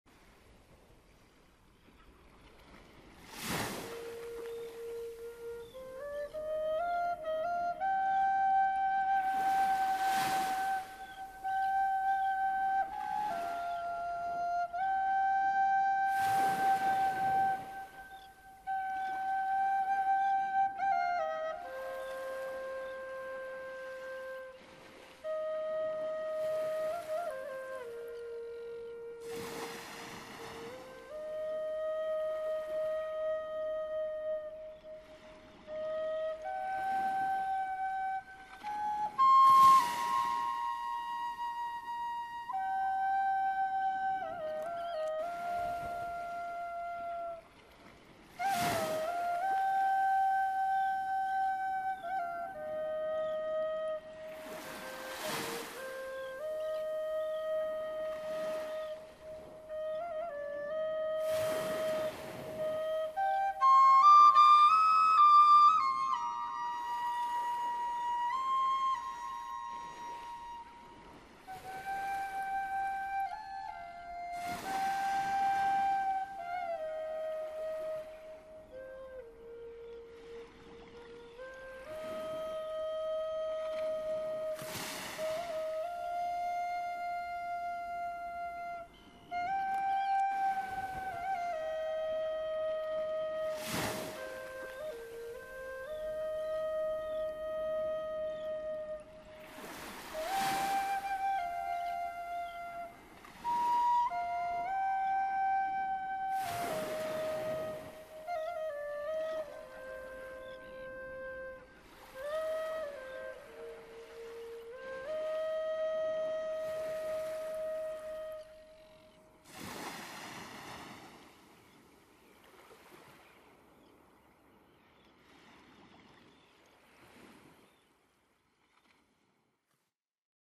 irish wooden whistle Bb, birch
made a little irish whistle a while ago.. its a Bb made of birch. locally hosted in northern Norway... well.. and yesterday we had some nice northern lights.. so here i put the two together...